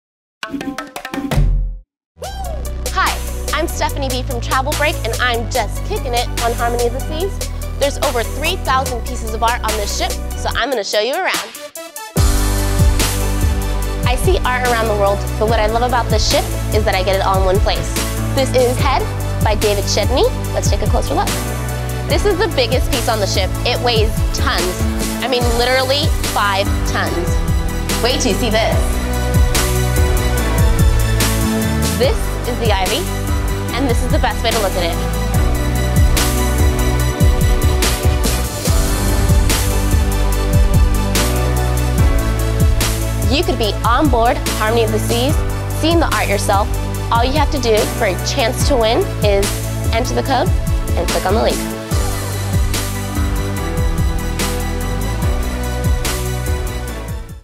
Harmony of the Seas: Choose Your Adventure | Art Tour
This is not your average art exhibit. This is an immersive, 'round every corner, installation adventure. Join Stephanie Be and discover unique art everywhere you turn on Royal Caribbean's Harmony of the Seas.